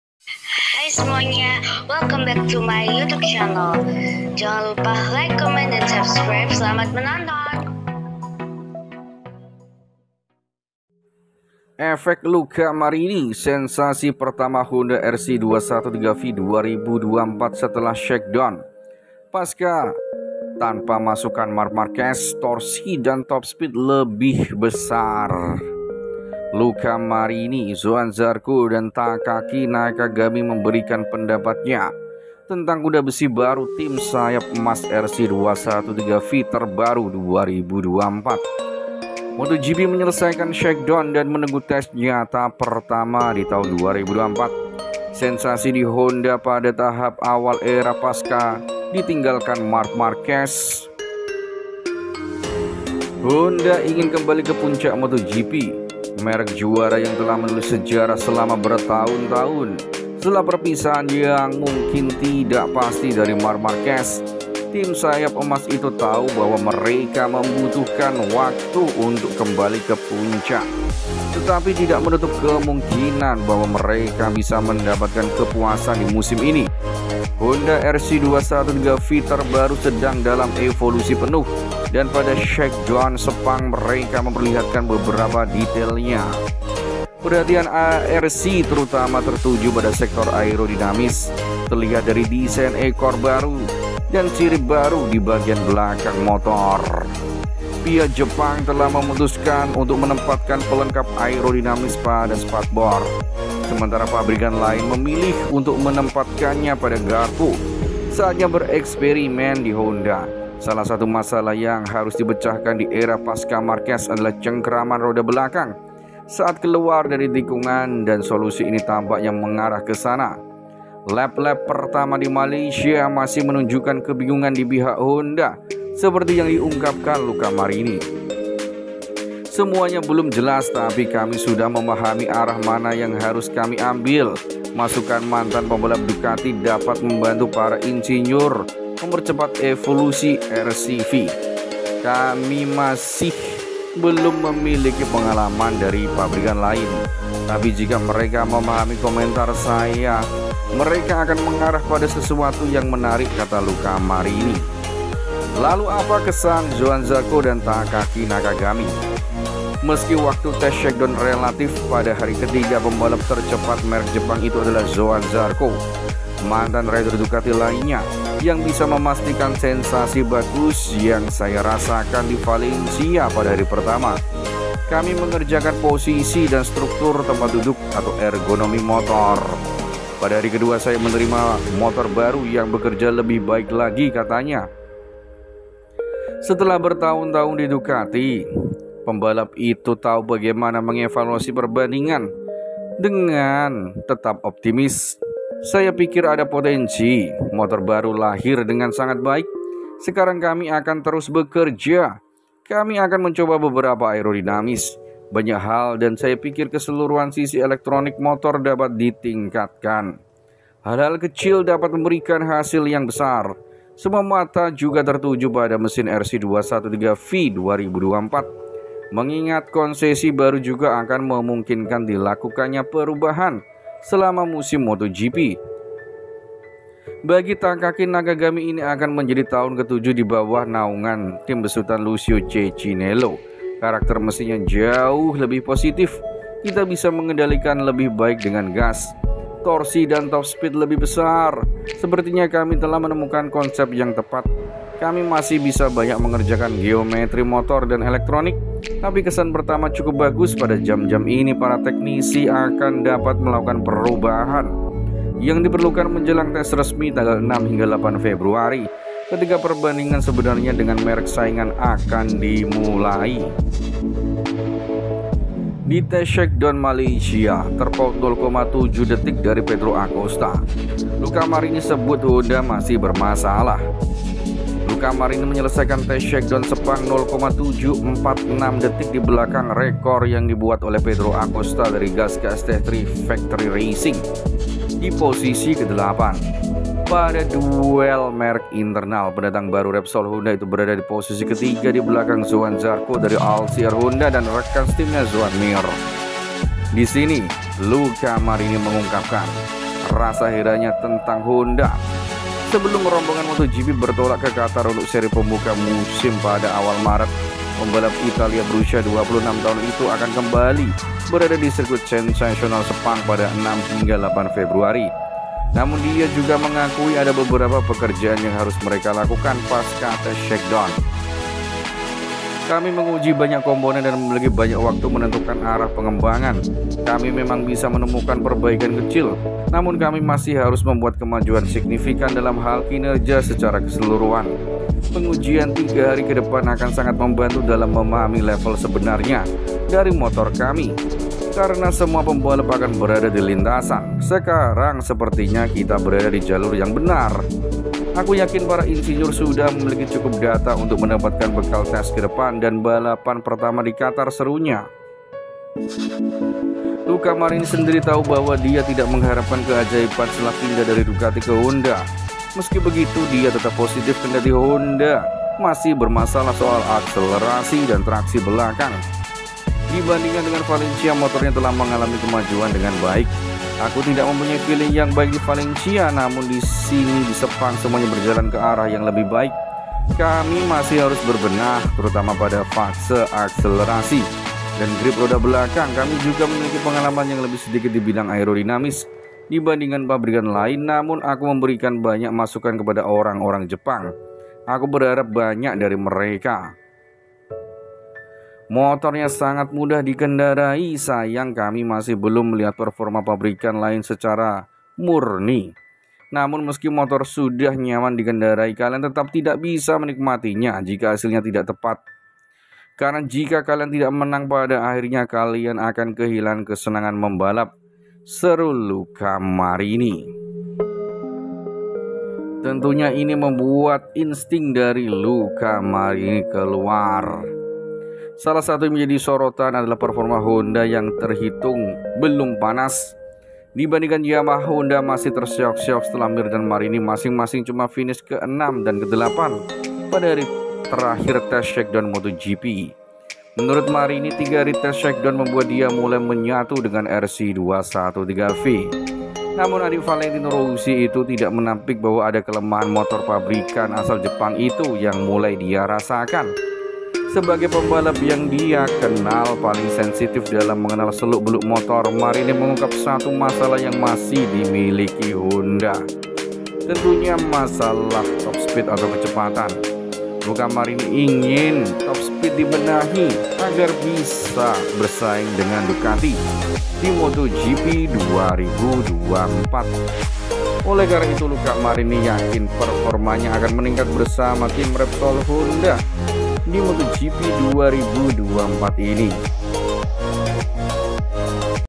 Super Agresif! Efek Luca Marini? Acosta Terpaut 0,7 detik ! Top Speed Honda RC213V 2024 Meningkat
Luca Marini menyelesaikan tes shakedown di Sepang 0,746 detik di belakang rekor yang dibuat oleh Pedro Acosta (GASGAS Tech3 Factory Racing) di posisi 8. Pada duel merk internal, pendatang baru Repsol Honda itu berada di posisi ketiga di belakang Johann Zarco (LCR Honda) dan rekan setimnya Joan Mir. Disini Luca mengungkapkan rasa herannya tentang Honda…